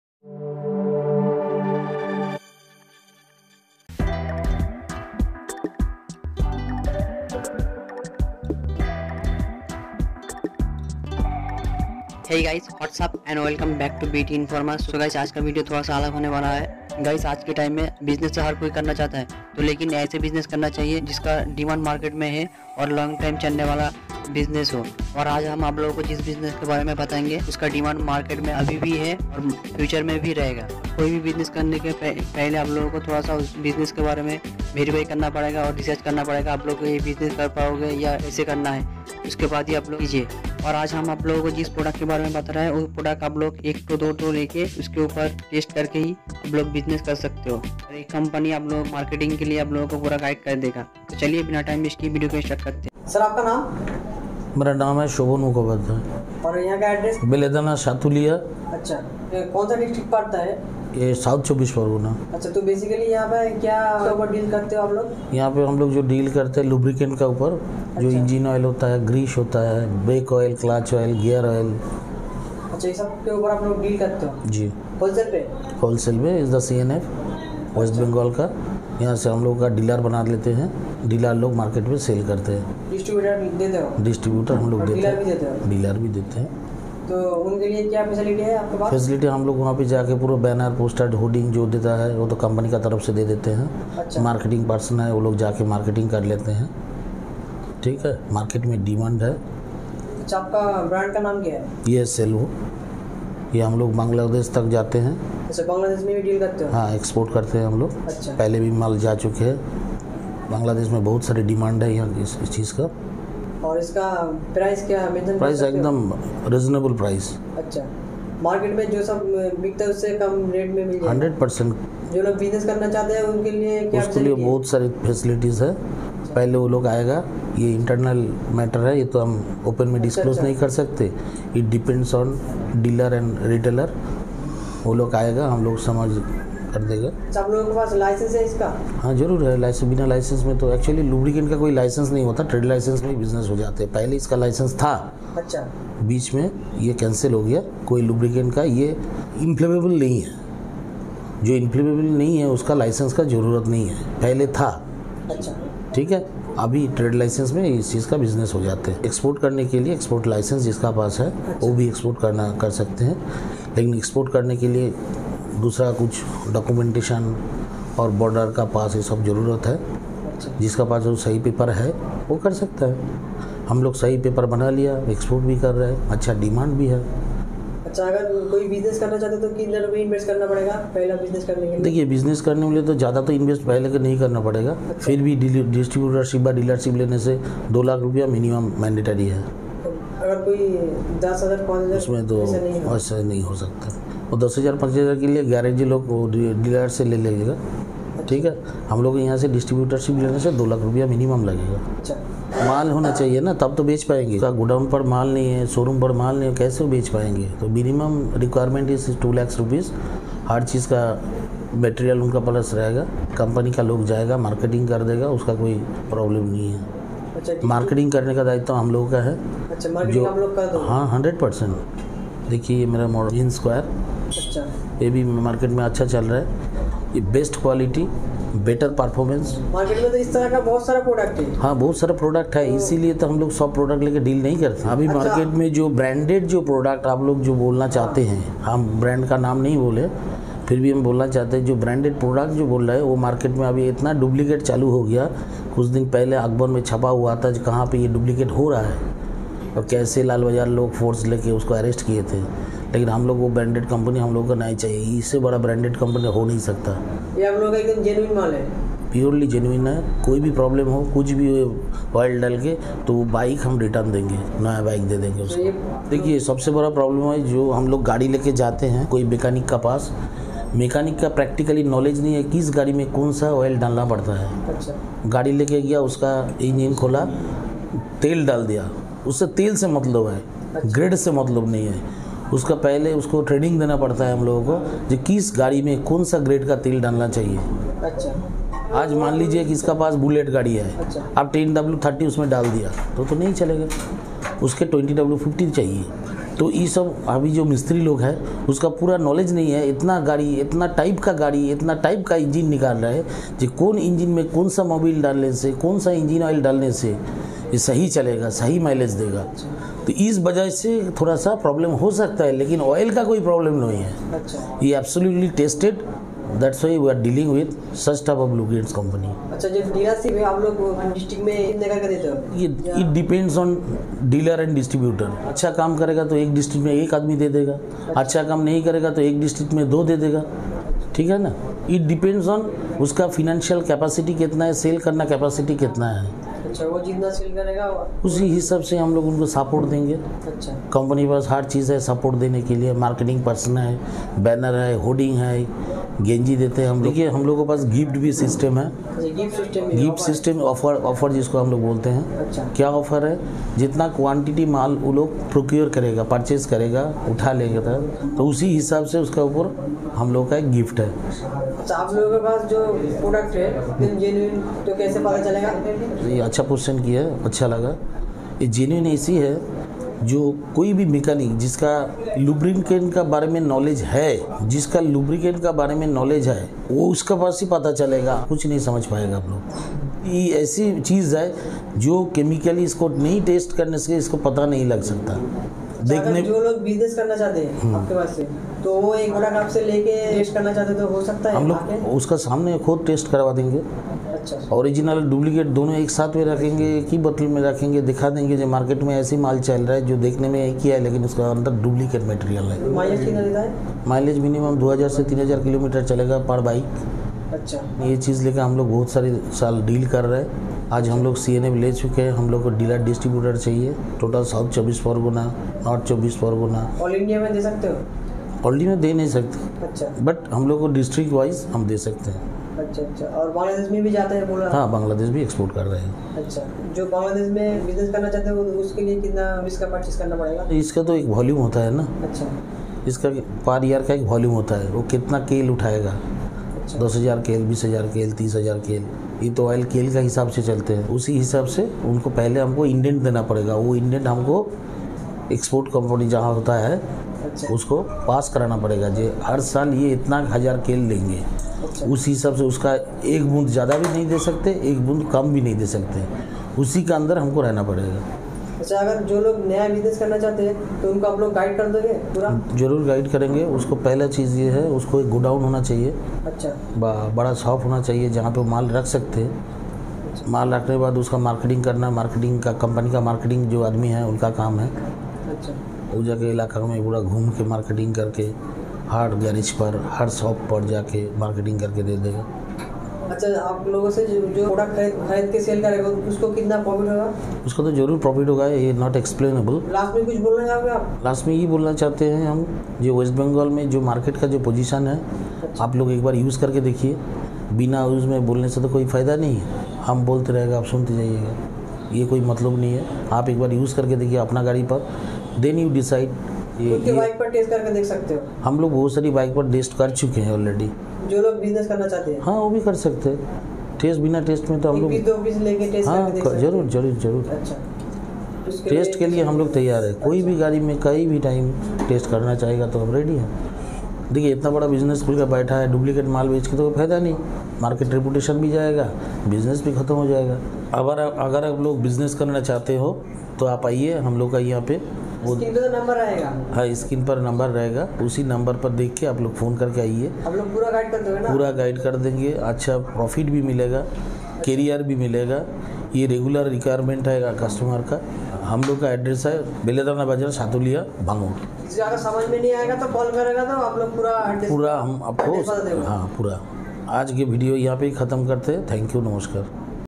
SAHARA LUB CENTRE | Ap Ke Elaka Main Ap Phale Start Karo Iss Business Ko | BT INFORMER
Shop Details :-

SAHARA LUB CENTRE

SELLVO  LUBRICANT  SG  PETROLEUM

Address :-

Vill - Konchpukur,  P.o - Krishnamati,  P.s - Kashipur,  Dist - South 24 Parganas